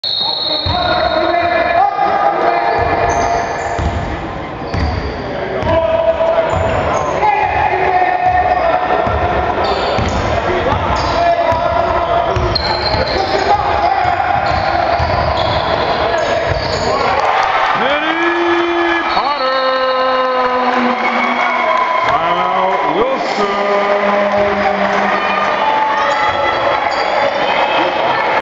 Kenny Potter Bides His Time, Makes His Move, Then Drives For The Win!
With little time left to spare and a tied score, San Pedro High boys basketball guard Kenny Potter was given the chance to deliver with the game on the line. Just like his magnificent 2011 football season, he would mightily deliver. Potter would use some time, then would blow by his man before driving to make this double-clutch layup with 2.3 seconds left and three defenders around him, giving the Pirates a dramatic 60-58 win over Wilson of Los Angeles in a Pirate Shootout Tournament quarterfinal from Wednesday, December 28, 2011.